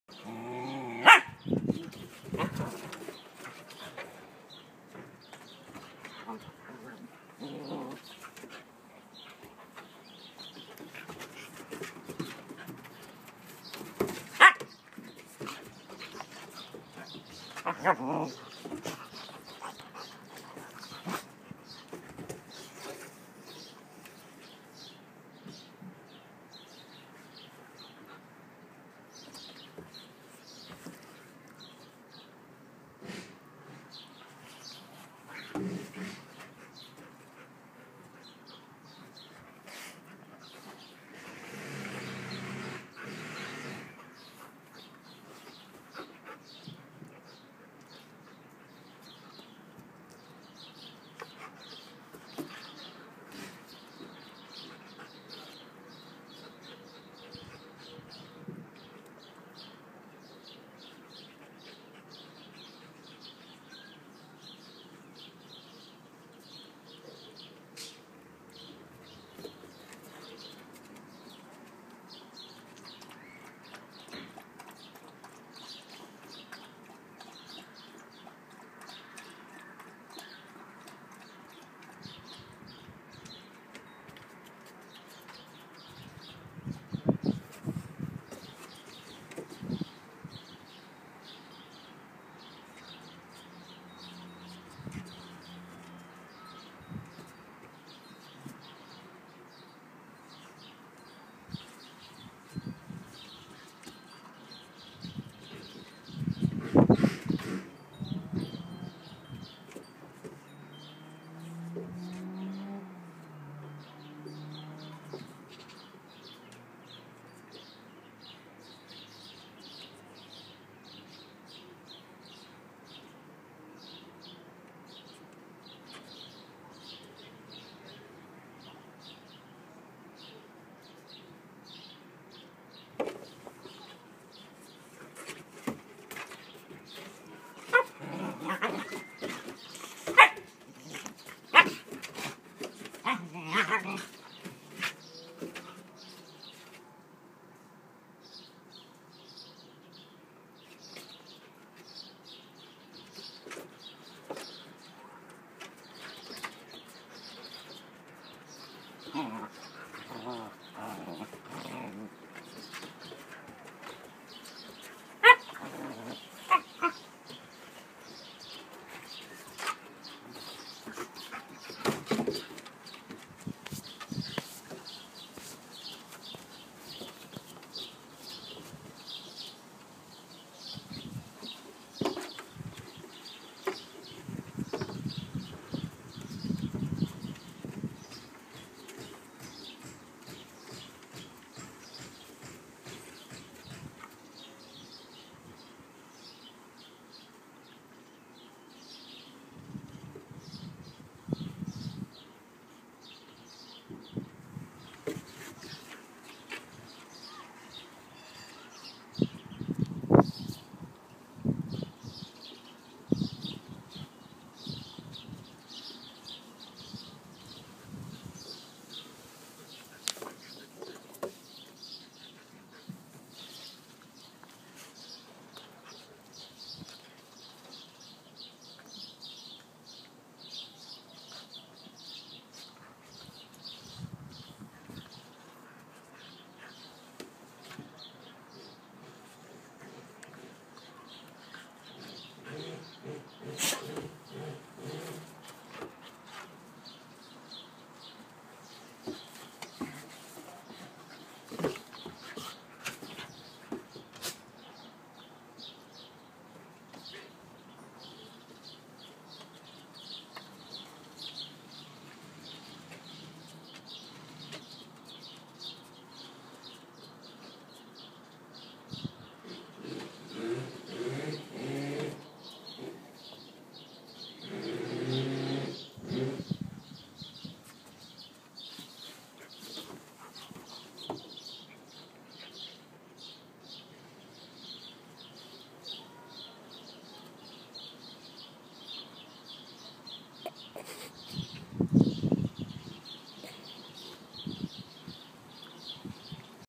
Husky play
Bella in this video is 4 months and Hunter is 8 months.

Bella is always loud in play and tends to be more agressive - just because she can get away with it. Hunter is more of a babysitter and let's Bella try her teething on his thick coat treating it as a massage.

When alone (unsupervised), Hunter would not allow Bella to be so much hyper.